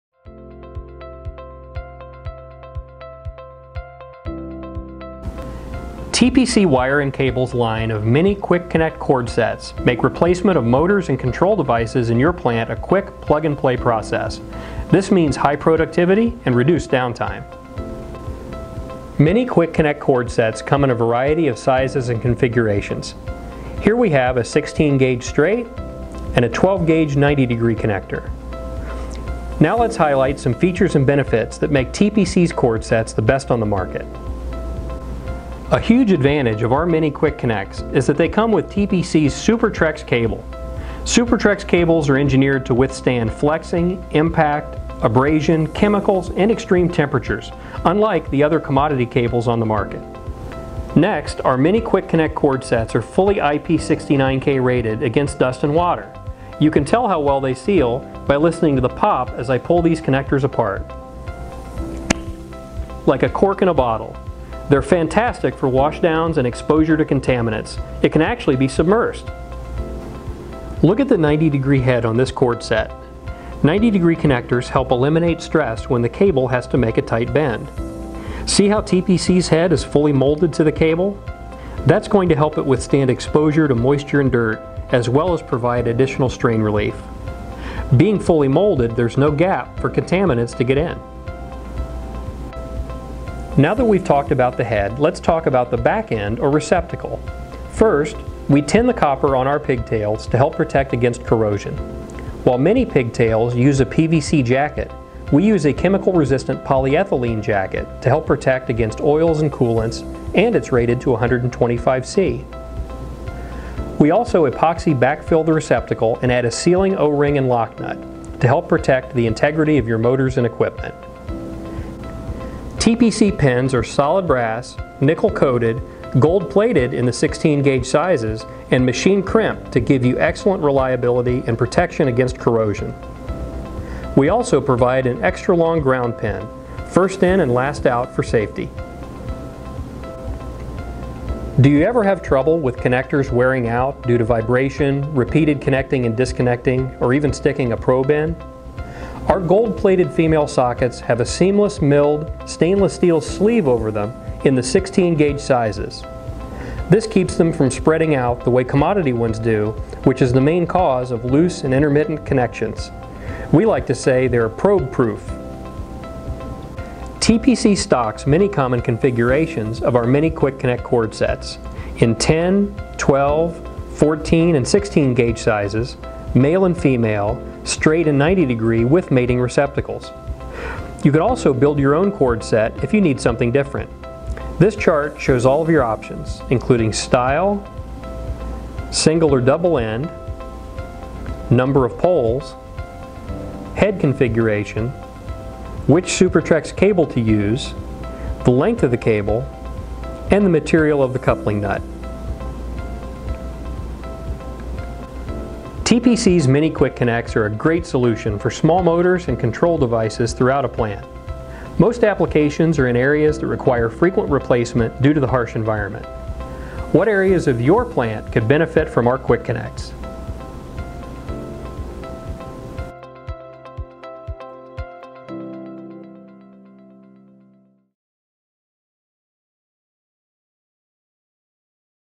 TPC Wire & Cable Quick-Connex™ Cordsets
Quick-Connex™ Cordsets are a collection of cord sets and cable assemblies that make replacing electrical and electronic control devices quick and simple. Plug sets are made with Super-Trex® SOO Ultra-Gard® Portable Cord which provides a superior first line of defense against tearing, abrasion, impact, oil, ozone, and most chemicals.

The cord is also flame and heat-resistant and offers extreme all-weather flexibility too. The molded and keyed vulcanized thermoset cord set provides rapid and secure connect and disconnect and ensures a water, oil, and dust-tight seal.